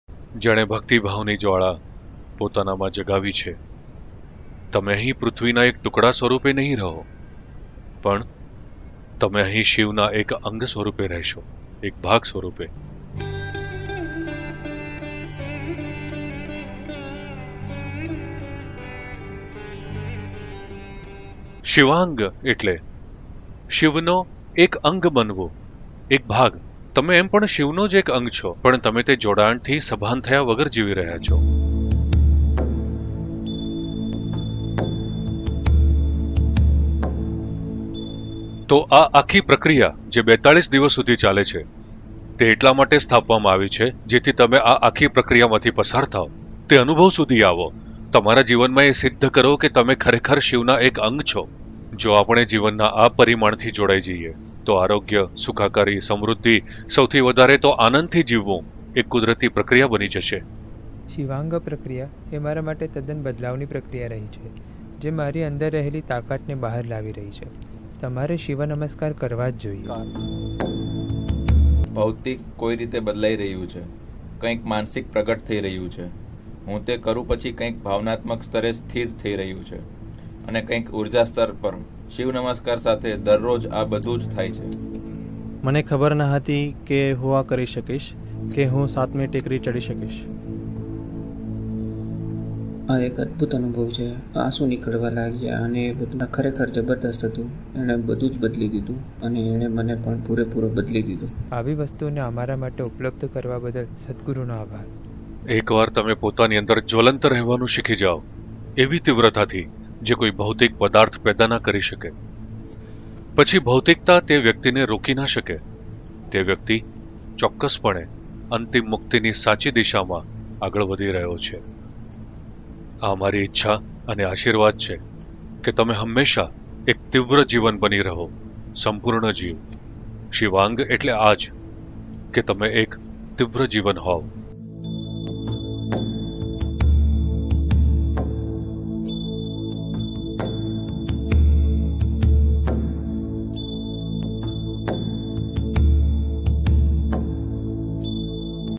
શિવાંગ : કૃપાનો માર્ગ | શિવનાં અંગ બનો | Shivanga - Way of Grace | Become a Limb of Shiva
શિવાંગ સાધના પુરુષો માટે પોતાની અંદરથી ભક્તિભાવ બહાર અવસર છે. શિવાંગ એટલે "શિવનું અંગ", શિવાંગ સાધના સર્જનના સ્ત્રોત સાથેના આપણા જોડાણ પ્રત્યે સભાન થવાનો એક અવસર છે.
આ ૪૨ - દિવસીય પ્રક્રિયામાં શક્તિશાળી શિવ નમસ્કાર પ્રક્રિયાની દીક્ષા અને દક્ષિણના કૈલાશ, વેલિયંગિરી પર્વતની તીર્થયાત્રાનો સમાવેશ થાય છે.